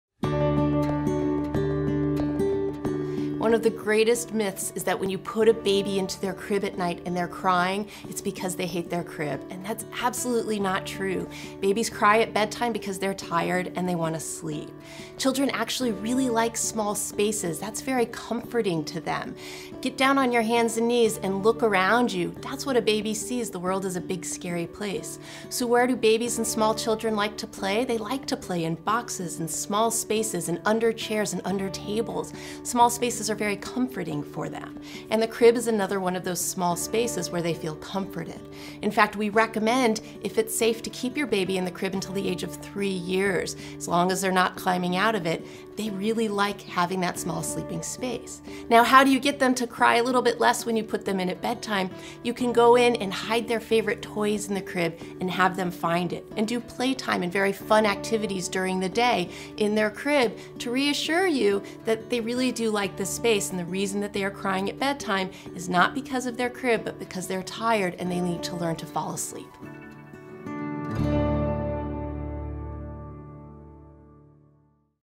My baby hates his crib. How can I change that? - Dr. Lisa Meltzer
One of the biggest myths is that when you put a baby into her crib and she’s crying it’s because she hates her crib. That’s absolutely not true! Babies typically cry at bedtime because they are tired and they want to sleep. Little ones actually really like small spaces, it’s comforting to them! As long as it’s safe and they are not climbing out, we actually recommend keeping them in a crib for sleeping until they’re three-years-old. To help her cry a little bit less at bedtime, try playing or doing a few pleasant activities in her crib during the daytime. For instance, you can hide her favorite toys in the crib and have her find them. This will also help reassure you that she does not cry at night because she does not like her crib but cries instead because she is tired and needs to learn how to fall asleep.